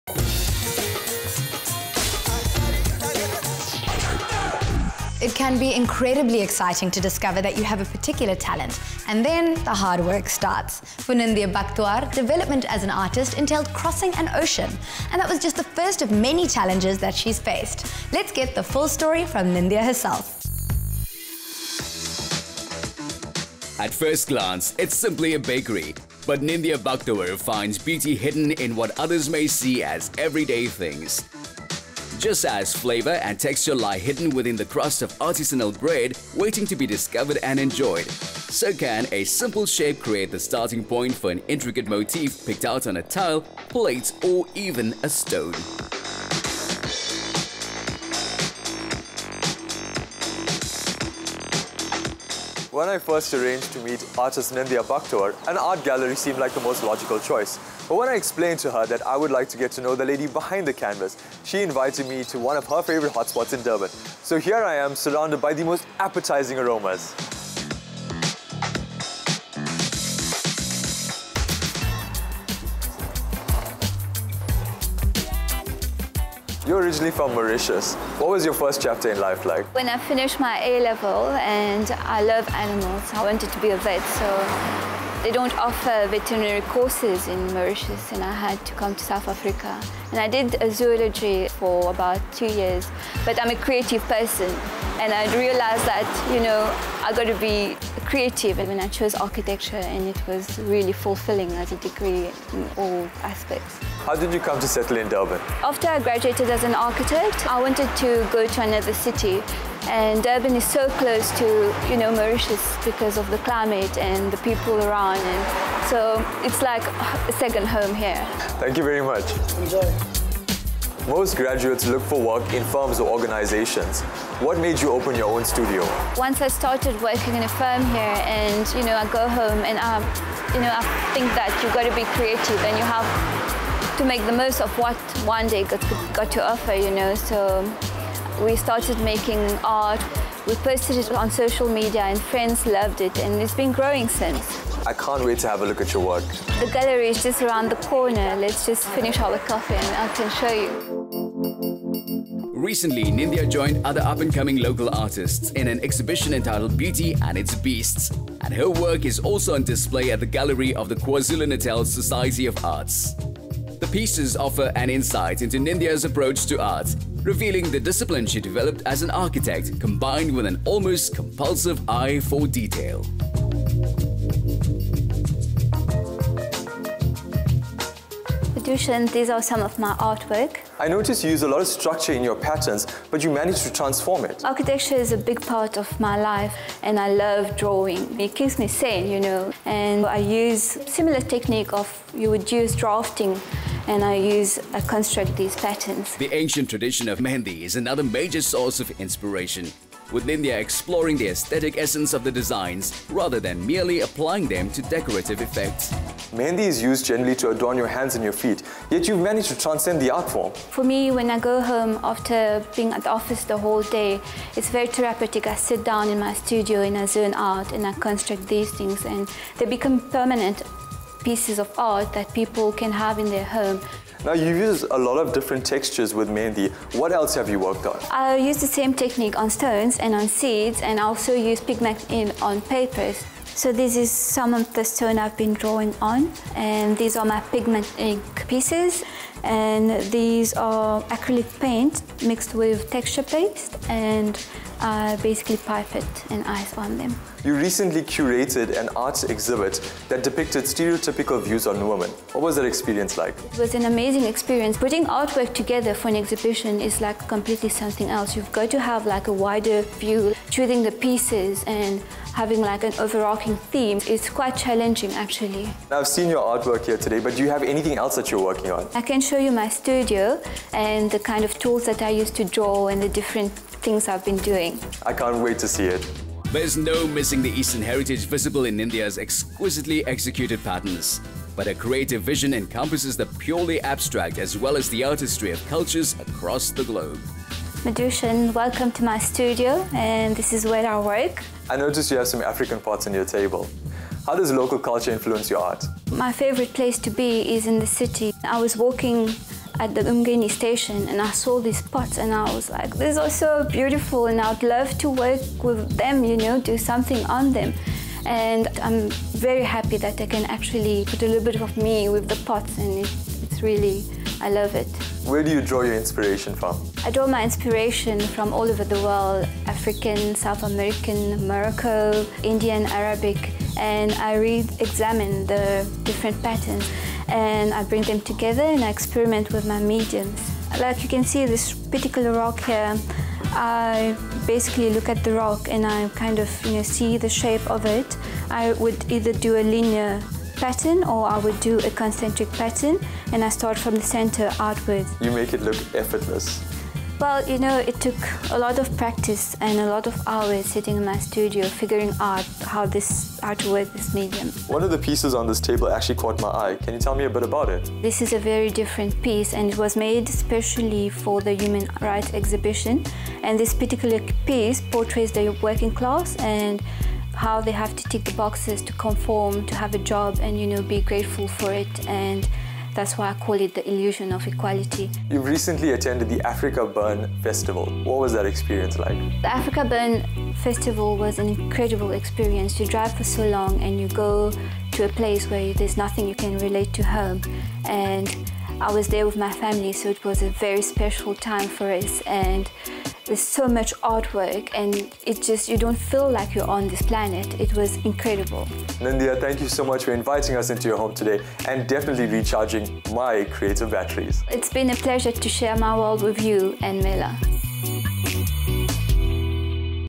Nindya Bucktowar : Artist
For Nindya Bucktowar, development as an artist entailed crossing an ocean – and that was just the first of many challenges that she has faced.  Madhushan met Nindya to get the full story.